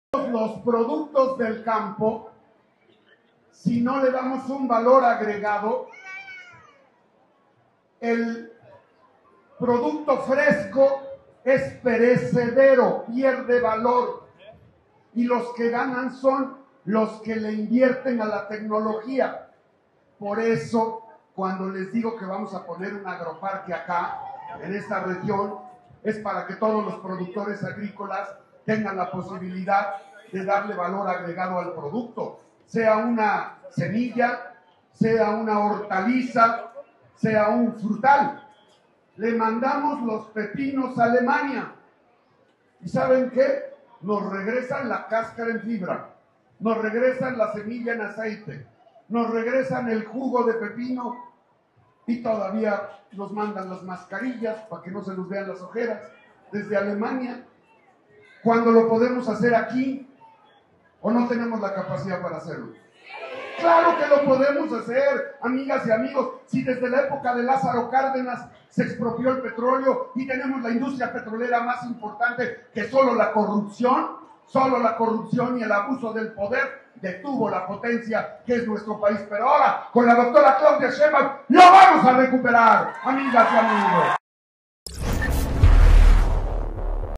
Con progreso y justicia, los agroparques transformarán el campo poblano: Alejandro Armenta
🧿El gobernador electo resaltó la importancia del reciente anuncio de la presidenta electa de México, Claudia Sheinbaum, de continuar el programa Sembrando Vida, subrayando que es un impulso crucial para el estado